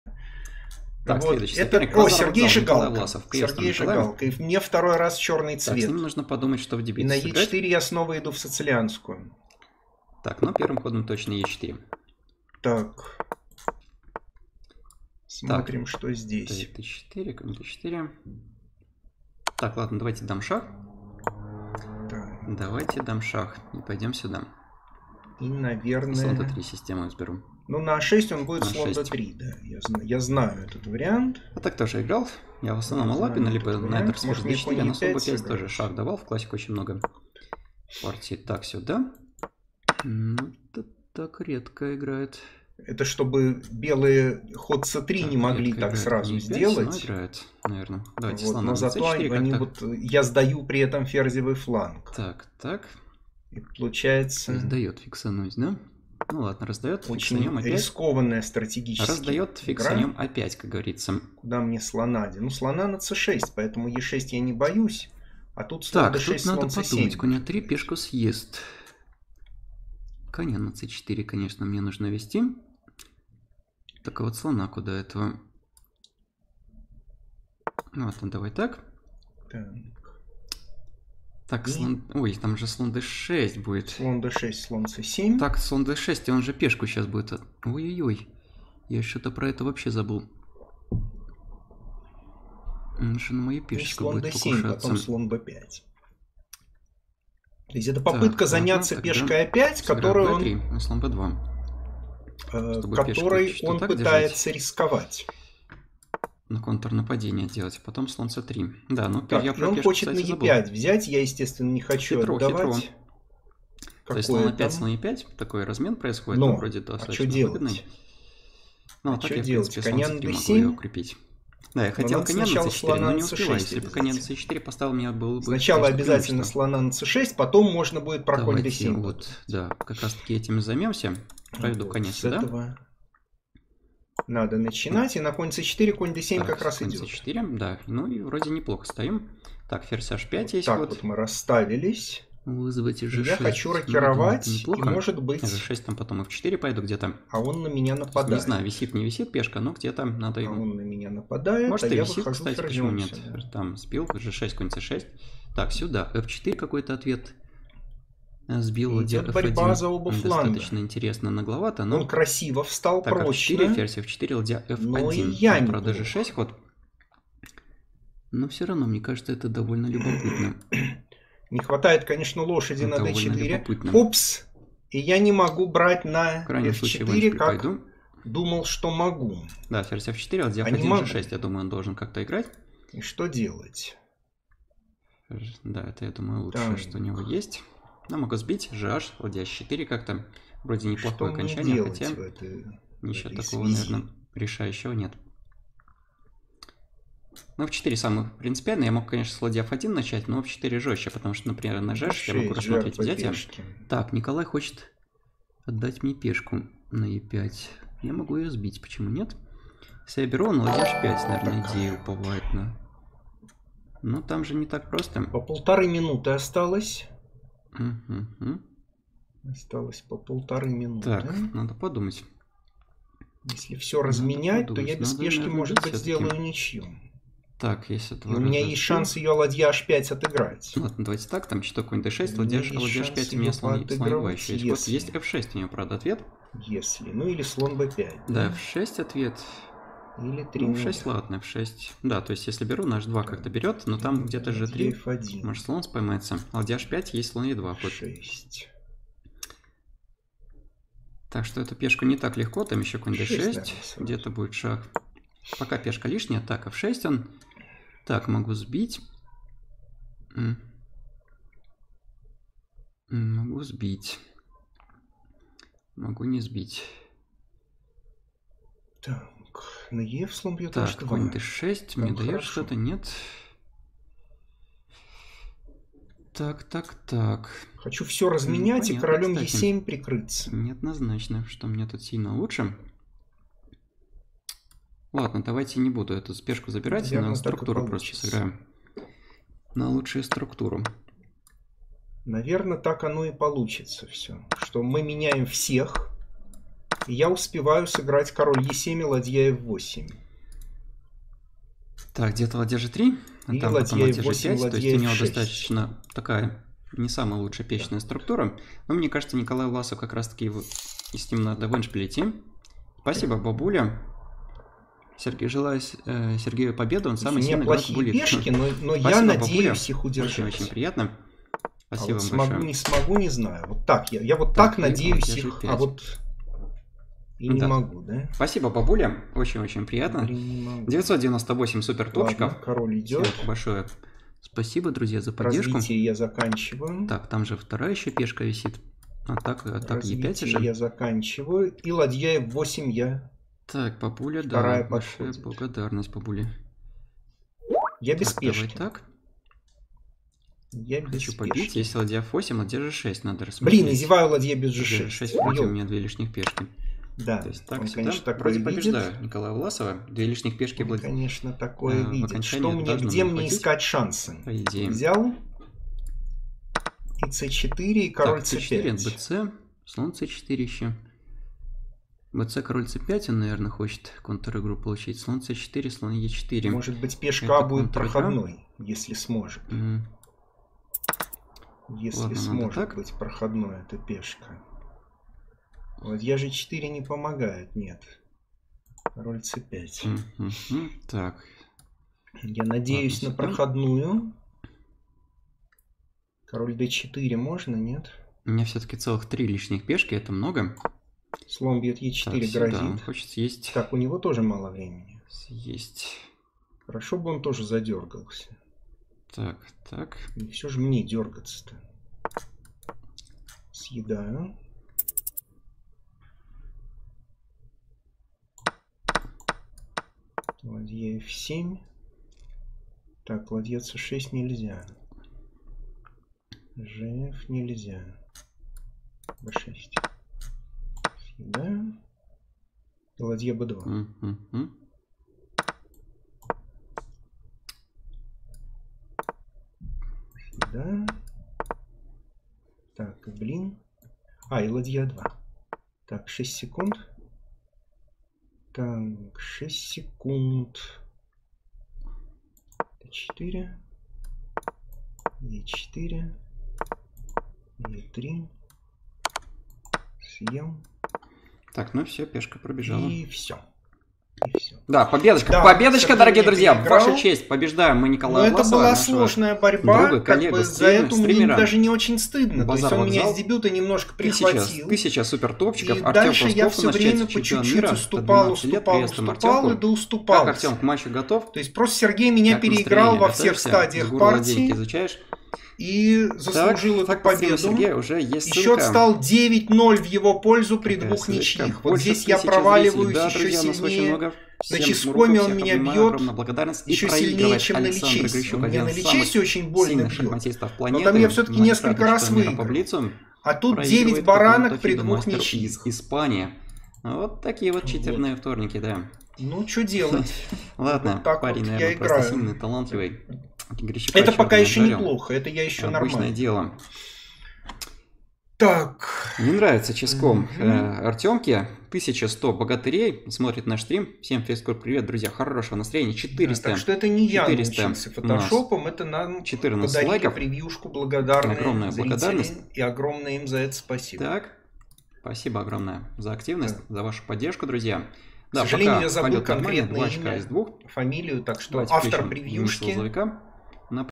ИДЕТ БОРЬБА ЗА ОБА ФЛАНГА! // GM СЕРГЕЙ ЖИГАЛКО vs IM НИКОЛАЙ ВЛАСОВ
TITLED TUESDAY 16/05/2023

bazar-wokzal chess
Международный мастер IM Николай Власов

Шахматы & Сергей Жигалко
Гроссмейстер GM Сергей Жигалко